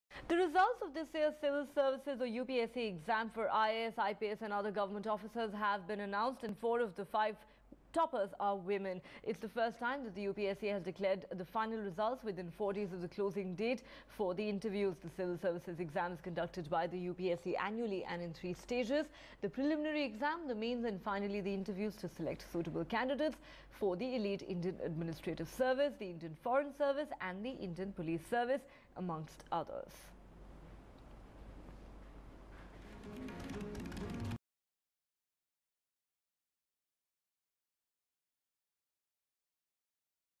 UPSC civil services results declared; 4 of 5 toppers are Women
"The Union Public Service Commission on Saturday declared results of the prestigious civil services examination.Ira Singhal has topped the exams.Four top position holders are women: Ira Singhal, Renu Raj, Nidhi Gupta and Vandana Rao.Suharsha Bhagat, at rank 5, is top male candidate in the list. Ira topped the exam in general category despite being physically handicapped. ""I want to be an IAS officer. I want to do something for the benefit of physically-handicapped people,"" she said.